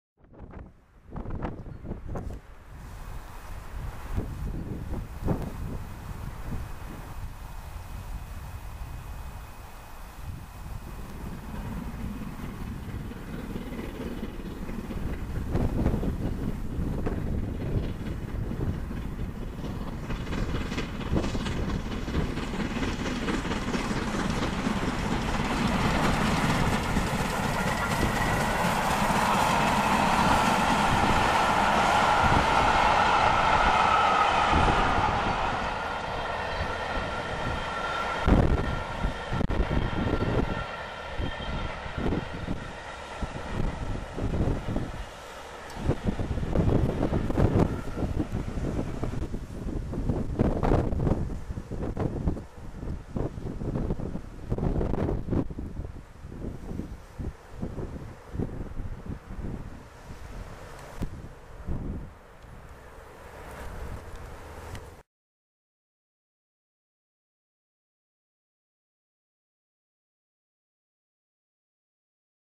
DOUBLEHEAD 44871+45407 THE CATHEDRALS EXPRESS 1Z44 5TH DECEMBER 2013 PASSING NEWSTON ST LOE BATH
LMS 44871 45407 THE CATHEDRALS EXPRESS 1Z44 5th December 2013

0.0  London Victoria  d 08:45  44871+45407
   Longhedge Jn  08:57
   Clapham Jn  09:01
   Hounslow  09:21
   Staines  09:32
   Addlestone Jn   09:50
   Woking  a 10:03
      d 10:06
   Guildford  a 10:16
      d 10:19
   Haslemere  10:33
   Petersfield  a 10:44
      d 10:56
   Havant  11:10
   Fareham E822  a 11:34
      d 11:44
   Hedge End  a 11:56
      d 12:10
   Eastleigh East Jn  a 12:18
      d 12:20
   Ronsey  12:34
   Salisbury  a 12:59
      d 13:19
   Warmisnter  13:41
   Westbury  a 13:49
      d 13:51
   Bradford Jn  14:01
   Bath Spa  a 14:17

Threat from development
Also read here on RAG against the road:

Mainline steam trains from England and other railway related videos from Poland, Australia, Italy and France.